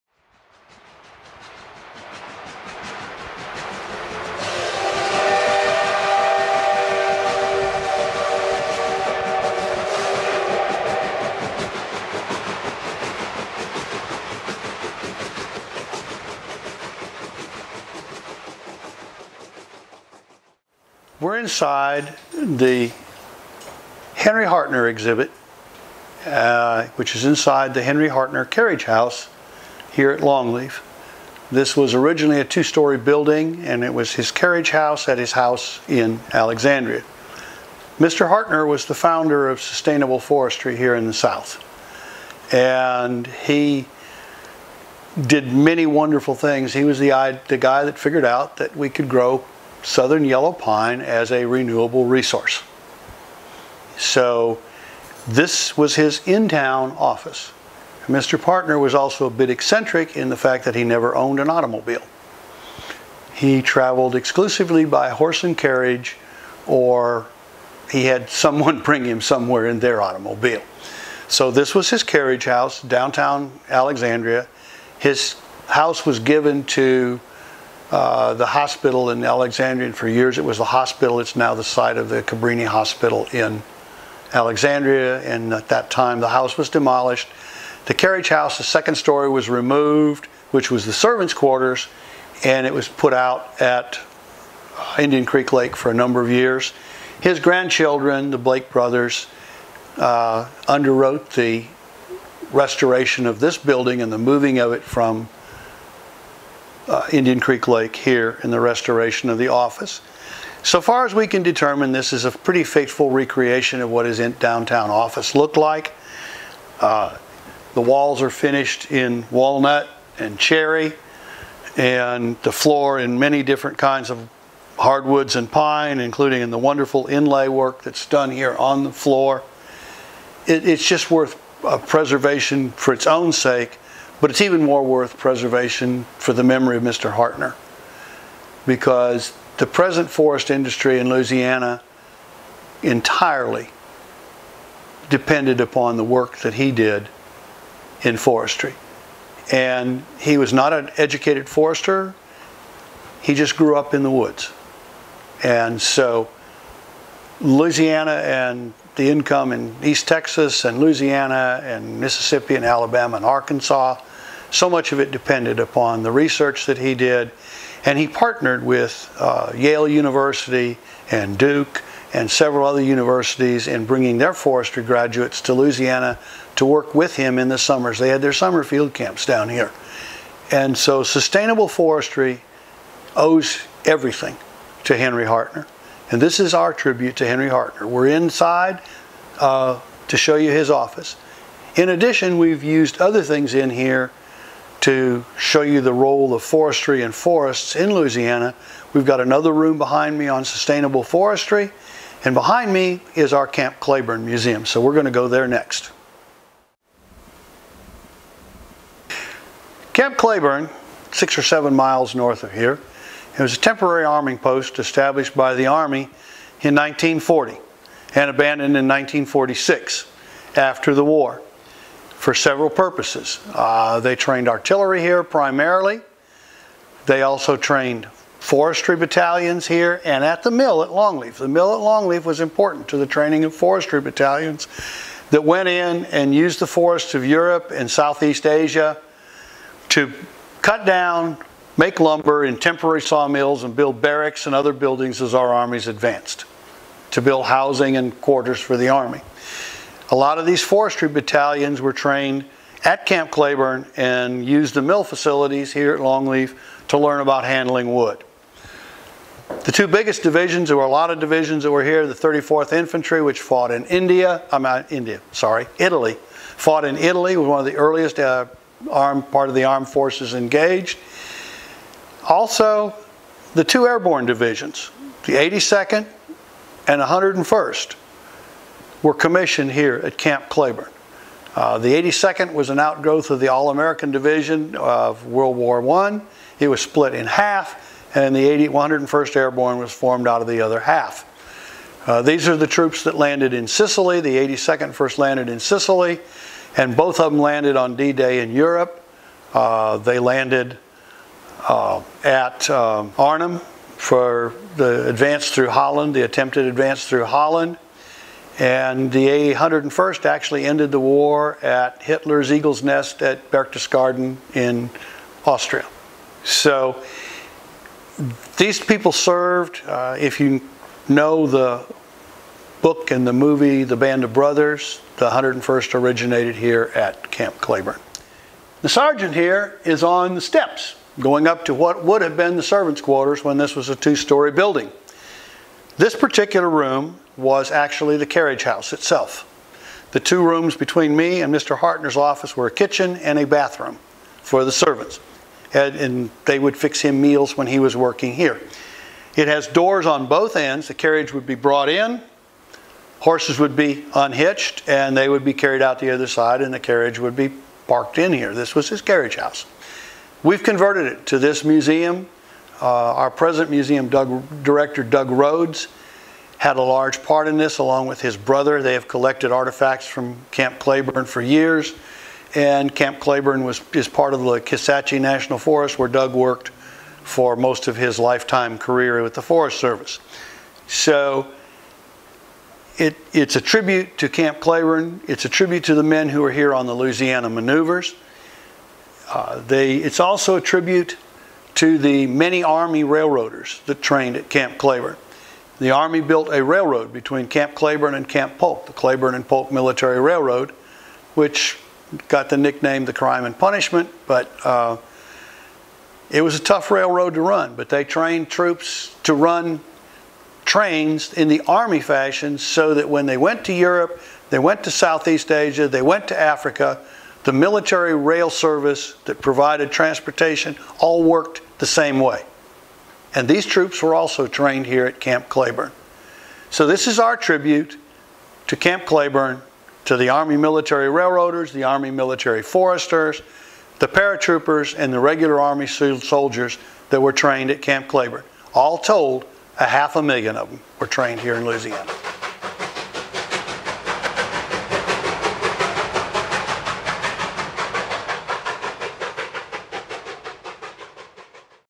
Long Leaf Historical Series: Episode VII - The Hardtner Carriage House & Camp Claiborne Museum
Hosted by Everett Lueck, this short documentary focuses on the history of the Hardtner House & Camp Claiborne Museum at Long Leaf, Louisiana - Southern Forest Heritage Museum.

Come visit Long Leaf... where time stands still.

LongLeaf.LA
RedRiverAndGulf.net

Video produced by RioGrandeExplorations.net